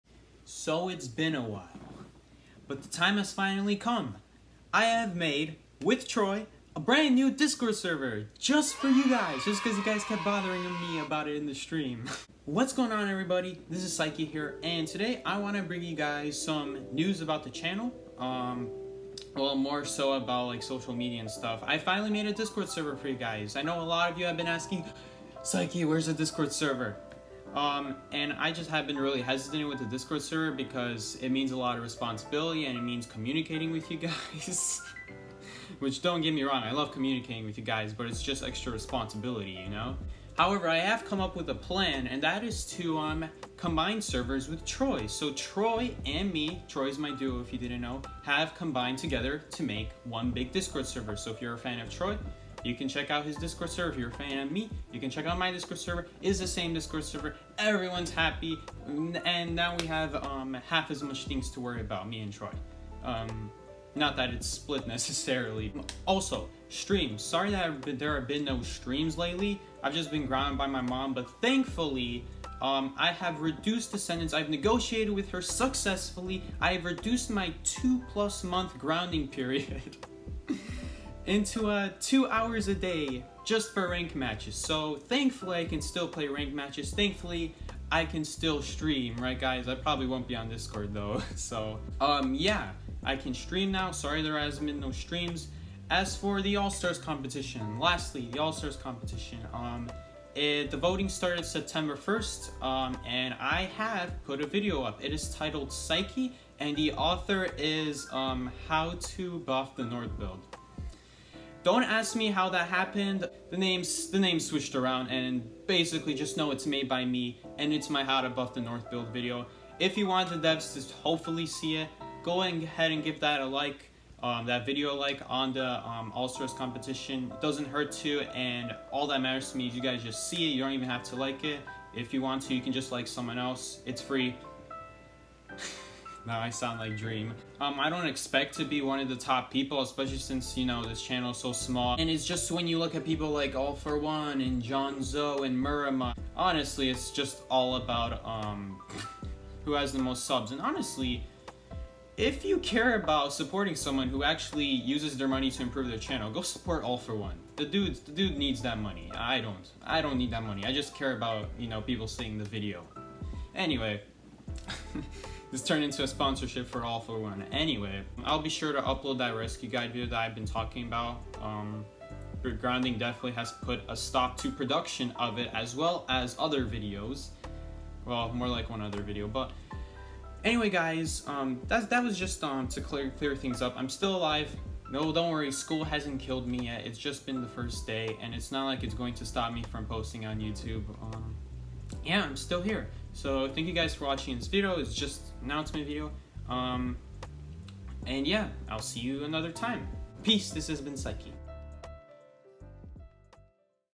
NEW DISCORD SERVER + Other Channel Announcements
And so finally here it is: A server that so many of you (or atleast I think so) have been waiting for you. It is mine and troi's great honor to present it to you.